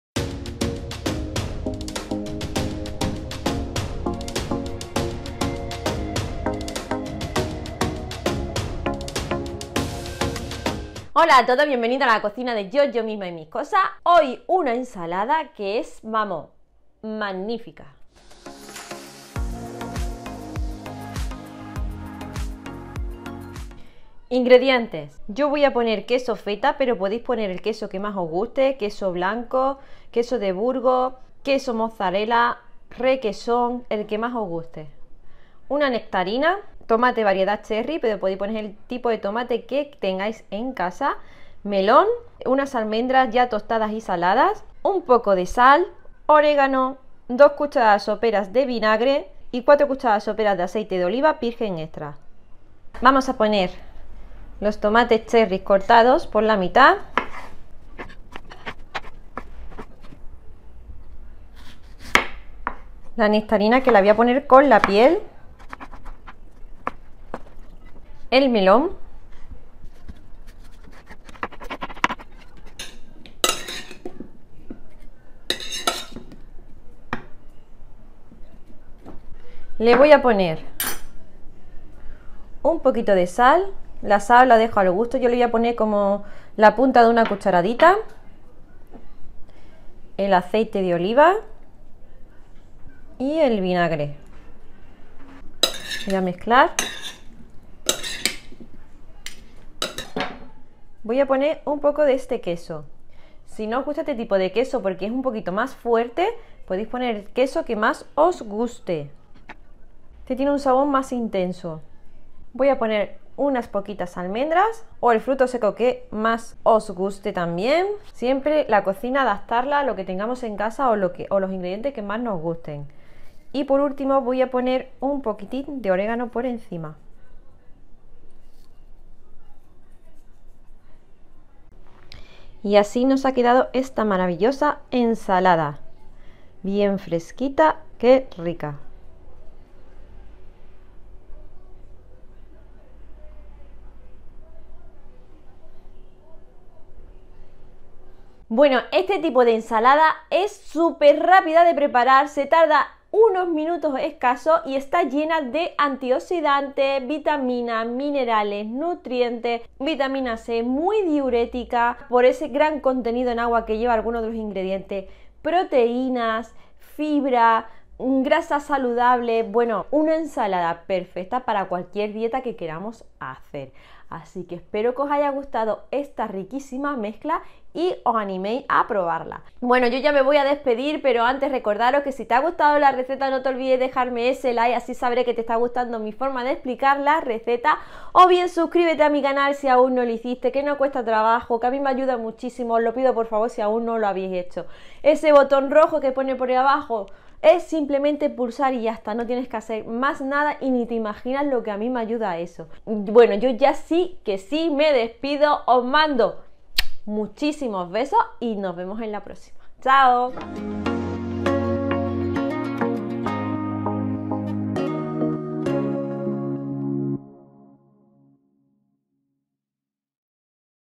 Como una SIMPLE ENSALADA ⭐ preparada en Minutos Te pueda Aportar tanto para tu Salud
Hoy como una simple ensalada preparada en minutos te puede aportar tanto para tu salud.

BENEFICIOS
Una ensalada rica en antioxidantes, vitaminas y minerales, proteínas, fibra, vitamina C, Calcio, muy diurética.

INGREDIENTES
Melón
Tomates, variedad cherry
Nectarina
Queso feta
Almendras tostadas
4 cucharadas de aceite de oliva
2 cucharadas de vinagre
Media cucharadita de las de café de sal
Una cucharadita de las de café a ras de orégano

AQUI OS DEJARE LA RECETA POR ESCRITO

Espero que os sirva esta rica idea de ensalada para dietas o ensalada saludable

Y si quieres ponerte en CONTACTO CONMIGO para consultas o para publicidad, puedes escribirme a mi correo electrónico

¡NOS VEMOS EN LA SIGUIENTE RECETA CORAZONES!